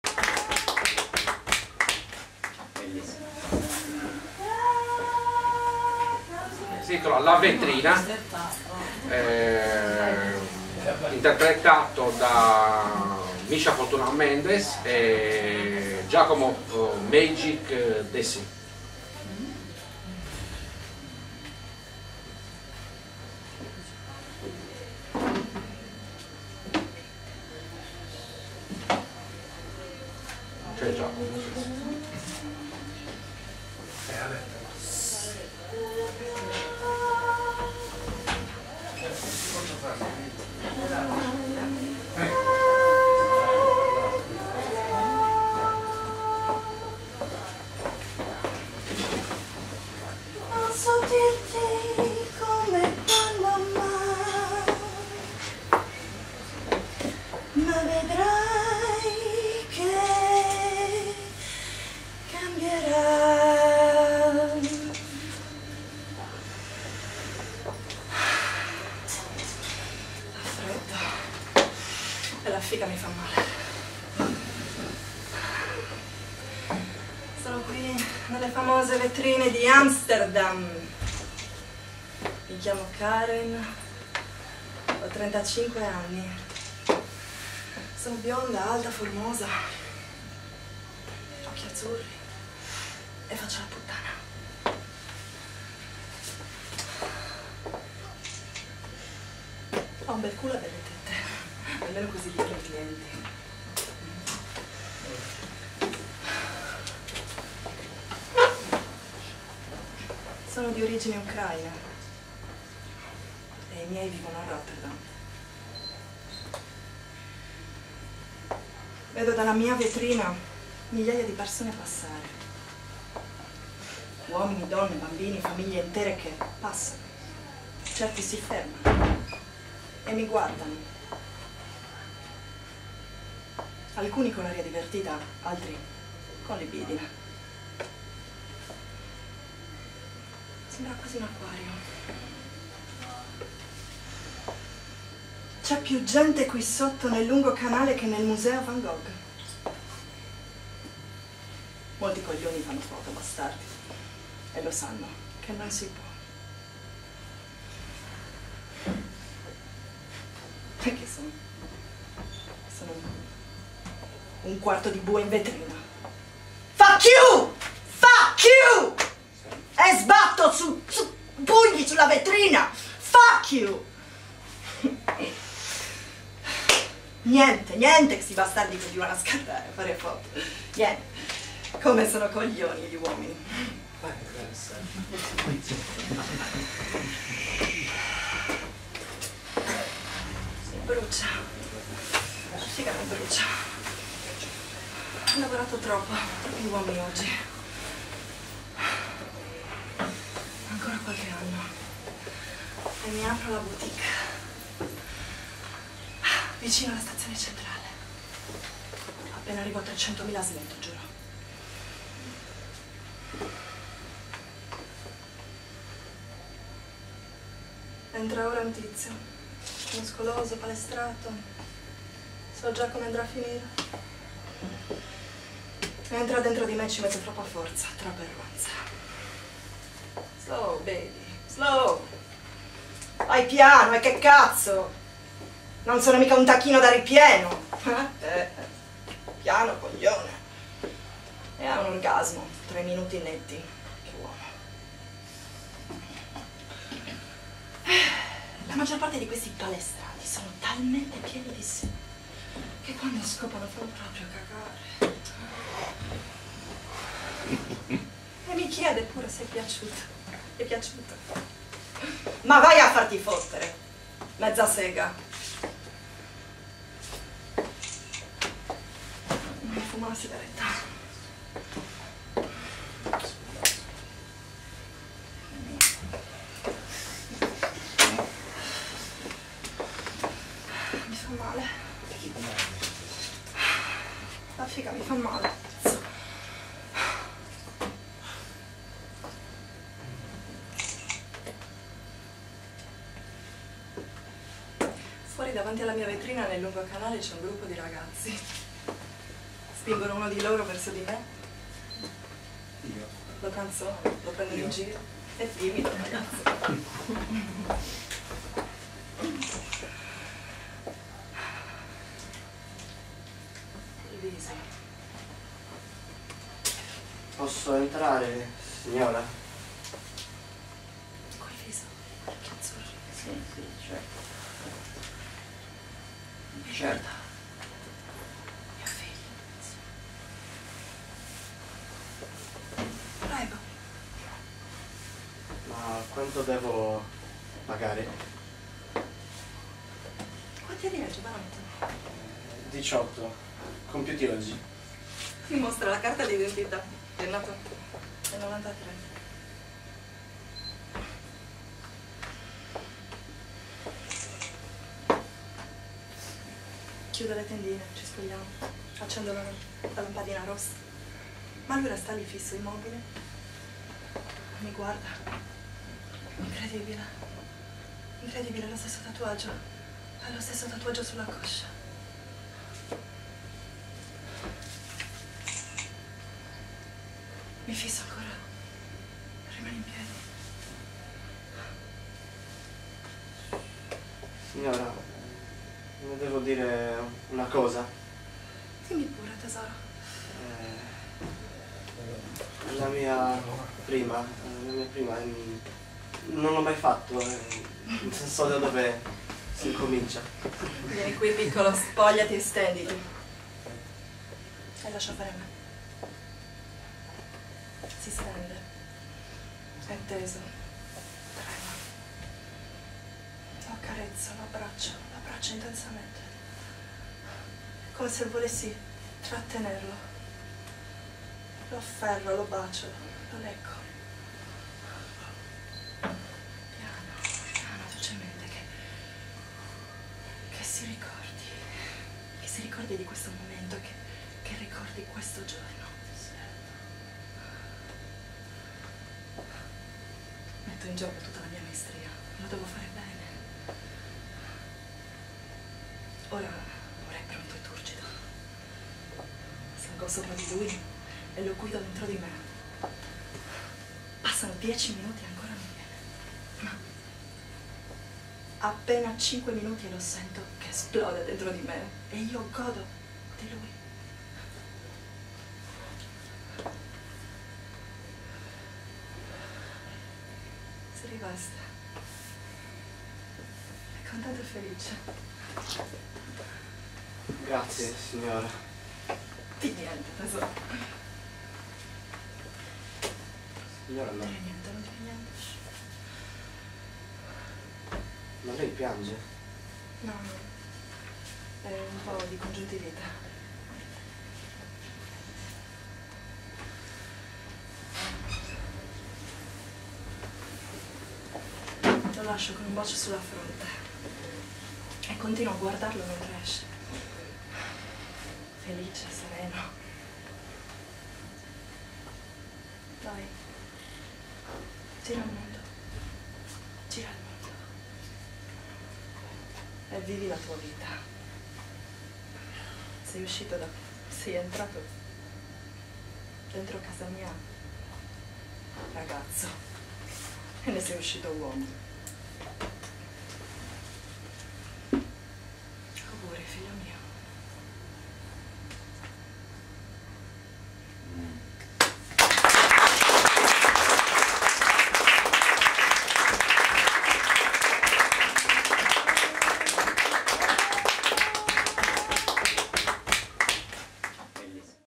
"" LA VETRINA" RACCONTO DI SANDRO MASCIA INTERPRETATO DALL'ATTRICE TEATRALE MISHA MENDEZ
racconto di Sandro Mascia interpretato e diretto dalla grande attrice Misha Fortuna Mendez, con la partecipazione del giovane attore Giacomo Dessi,